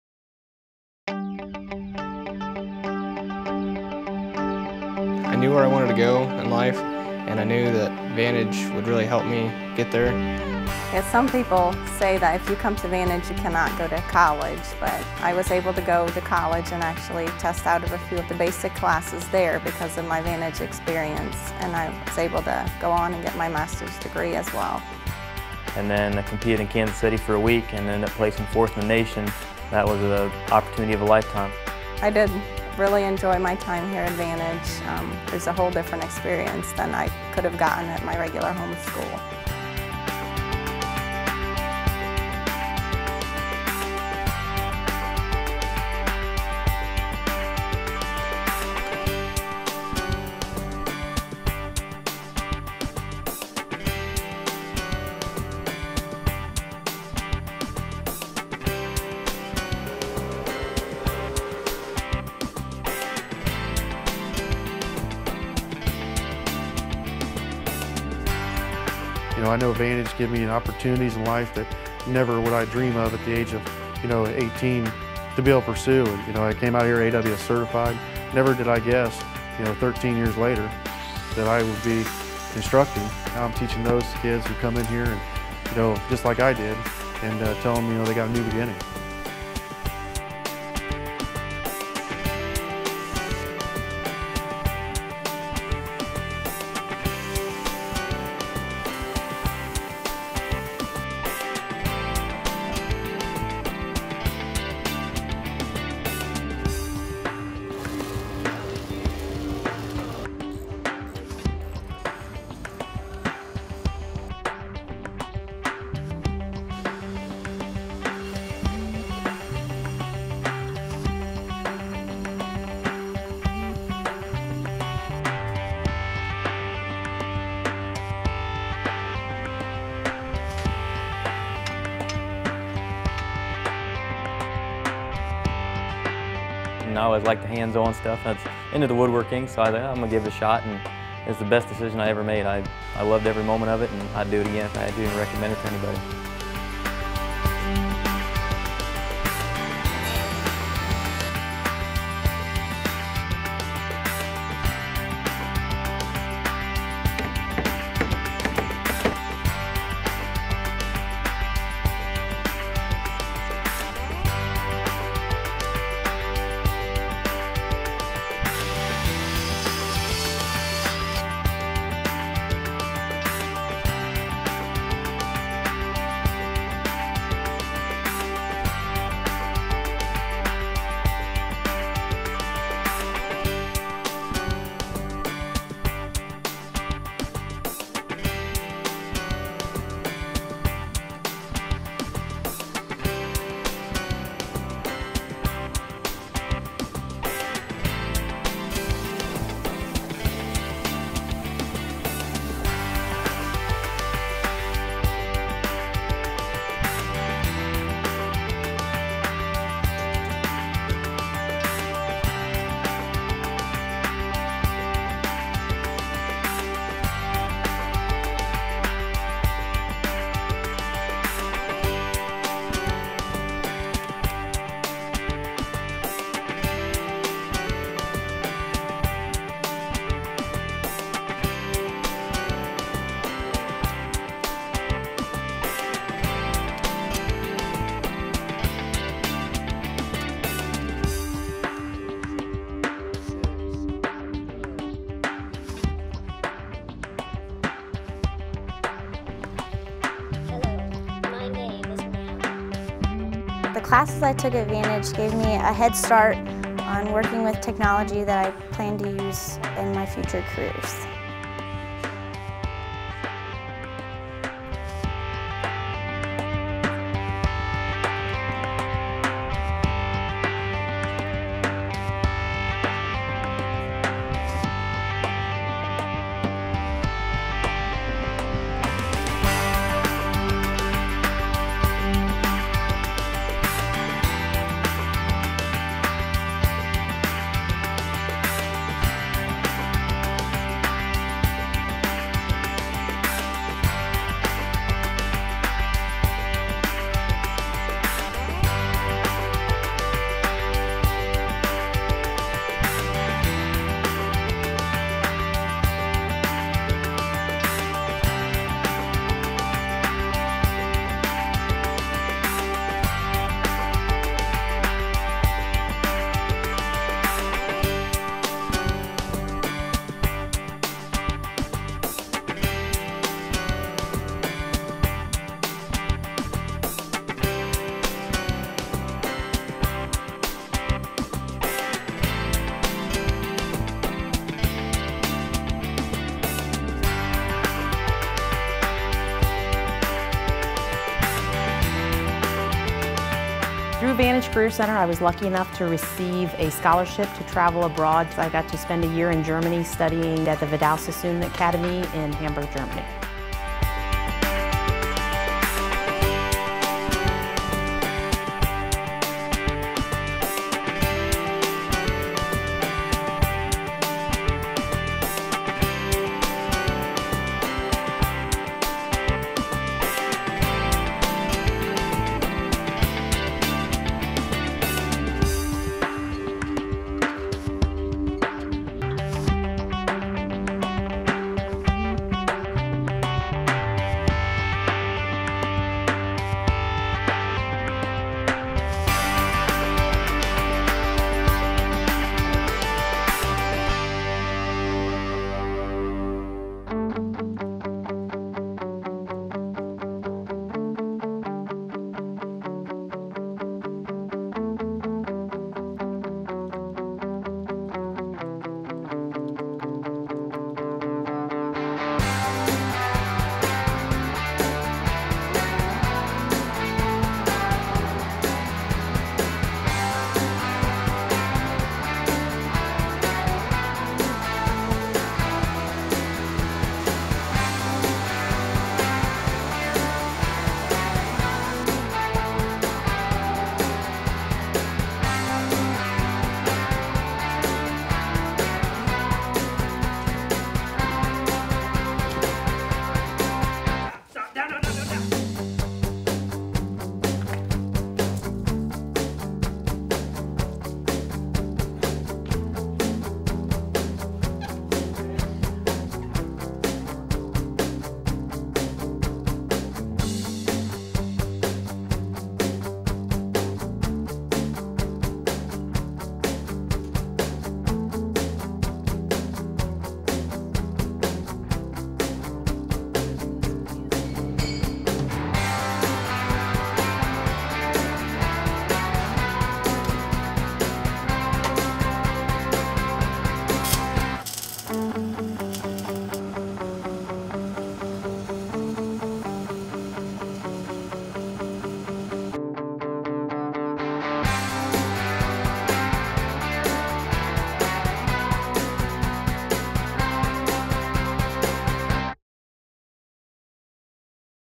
Vantage Career Center - Full Program Video 2013
Video highlighting all of the programs offered for the 2012-13 school year at Vantage Career Center in Van Wert, Ohio.  It includes alumni testimonials, high school, and adult programs.  This video is used for public presentations to give a sense of the breadth of programs and a look into the state-of-art classrooms and labs.